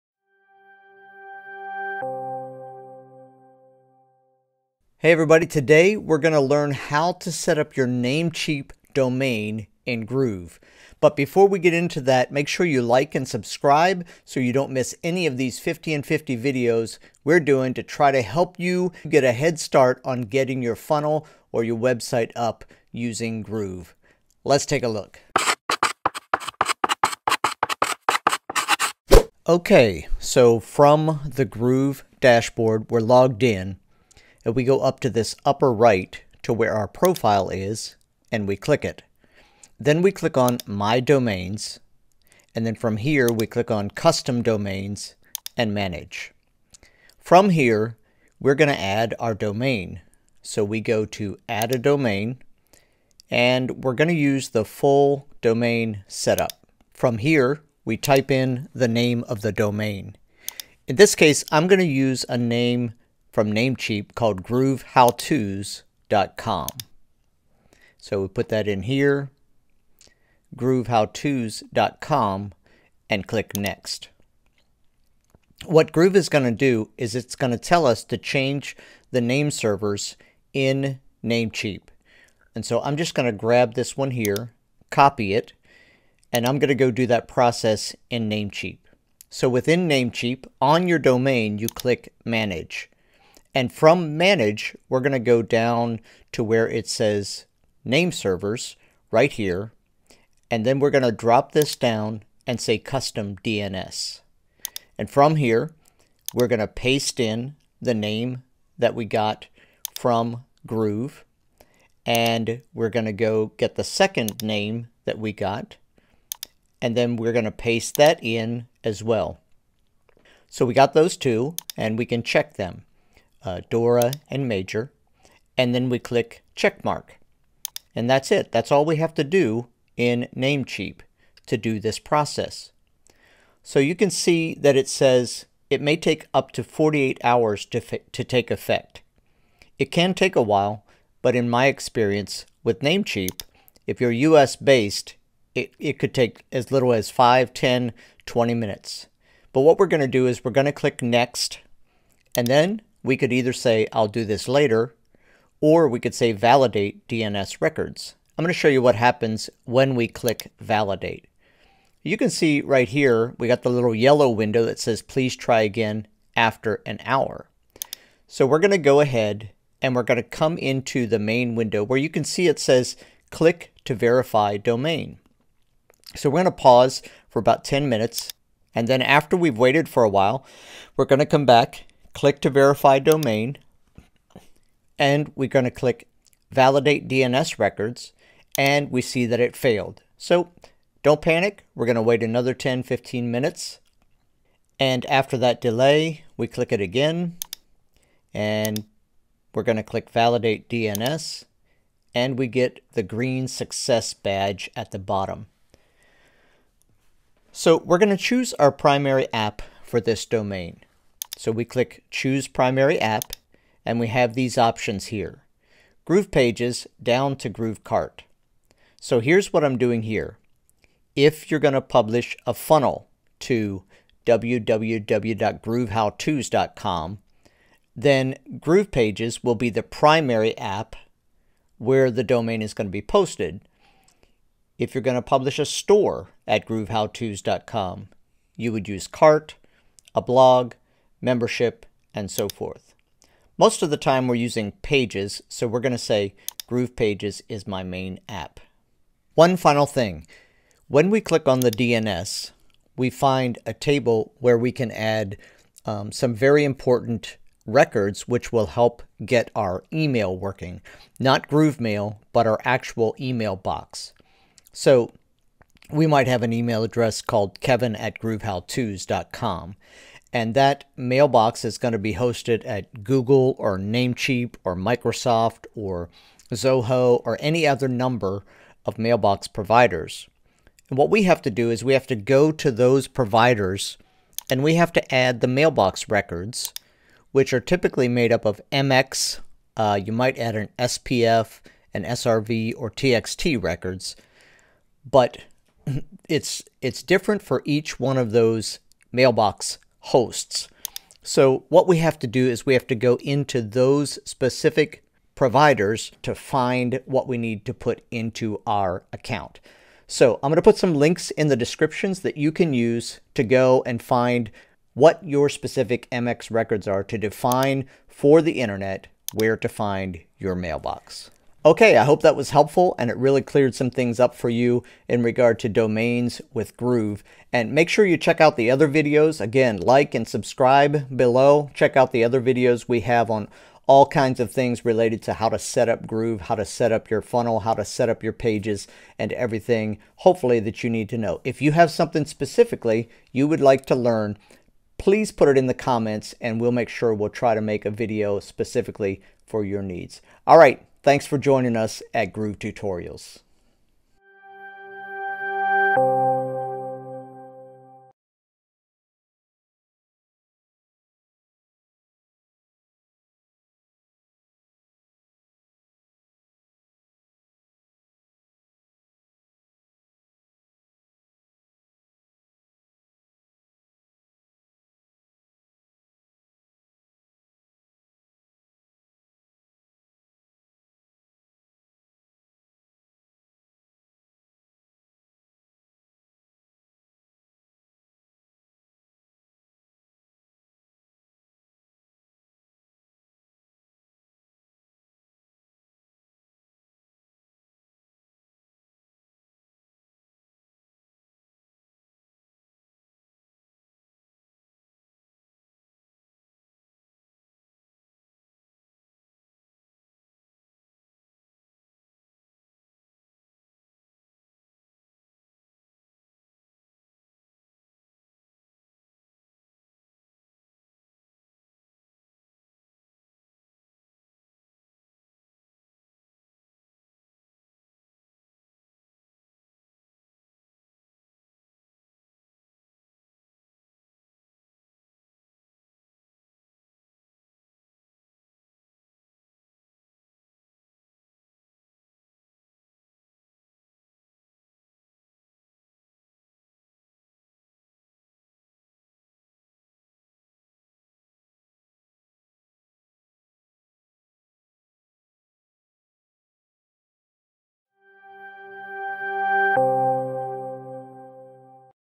NEW: Learn How to Setup Your Namecheap Domain in Groove
Now you can learn how to setup your namecheap domain in groove. So, you have begun building a funnel, website, store ... you name it ... in Groove. Now you want to setup your own custom namecheap domain so you can publish to it. Here's how.